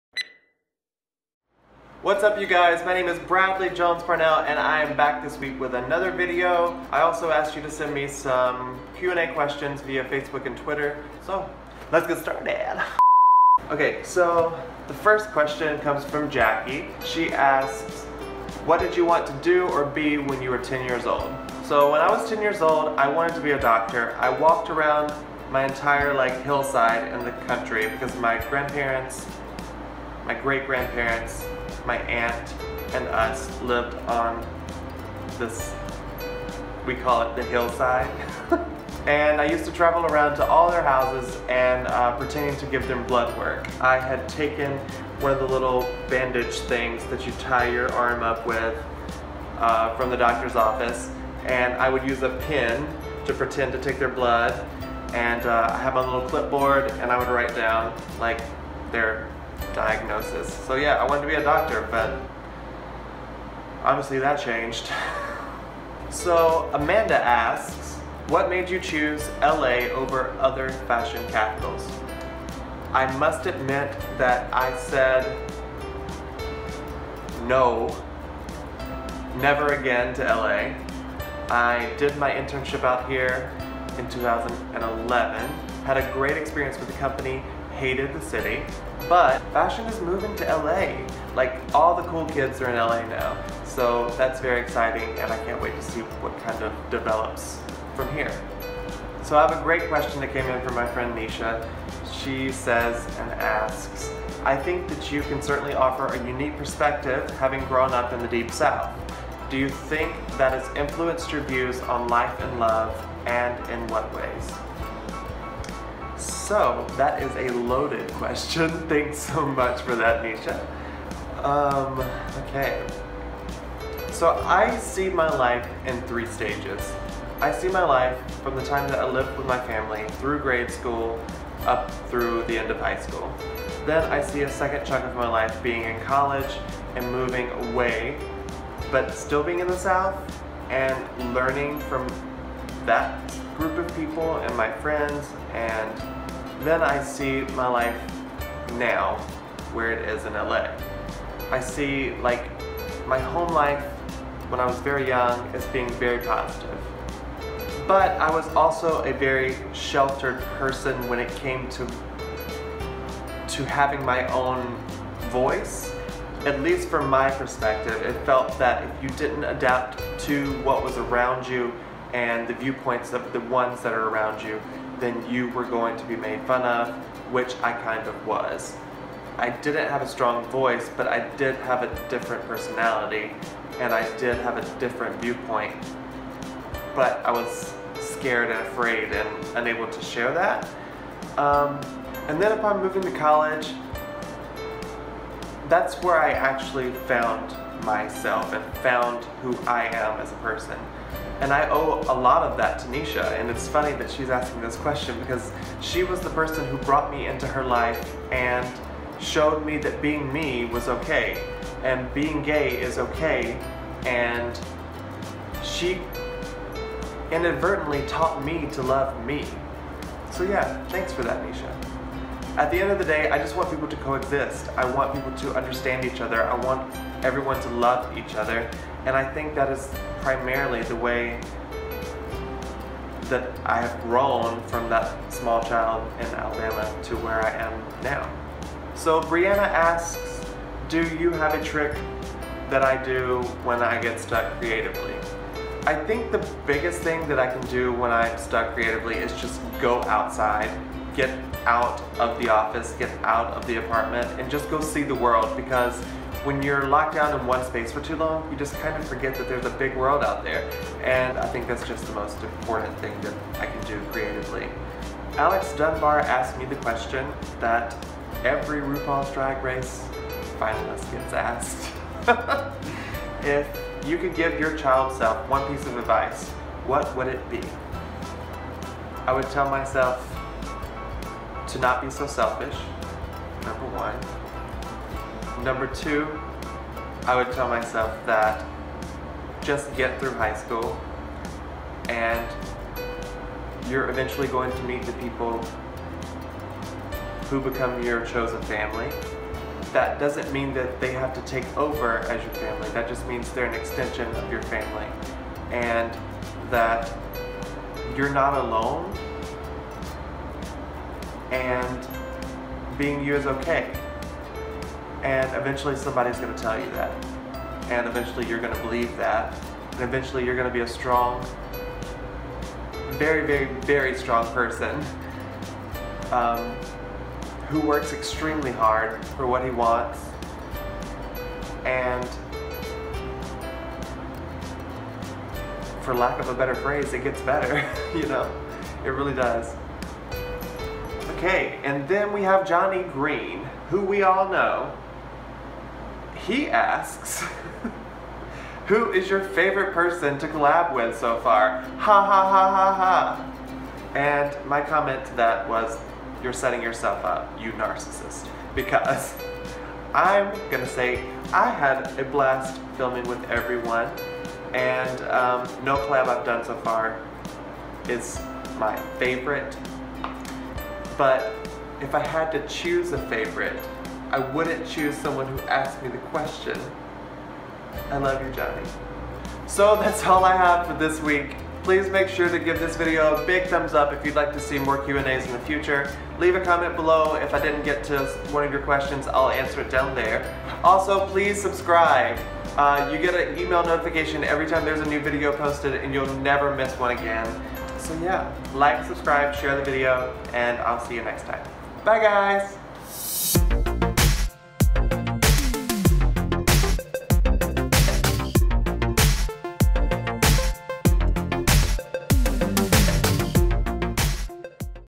Q&A Get's A Little Deep | Bradley Jones-Parnell
I asked my friends to send me questions for a little Q&A catchup. If I didn't get to yours, I tried... But ask me in the comments below and I will definitely answer them there!
• Music •
Hope you enjoyed the video! Have a great week! Hugs :)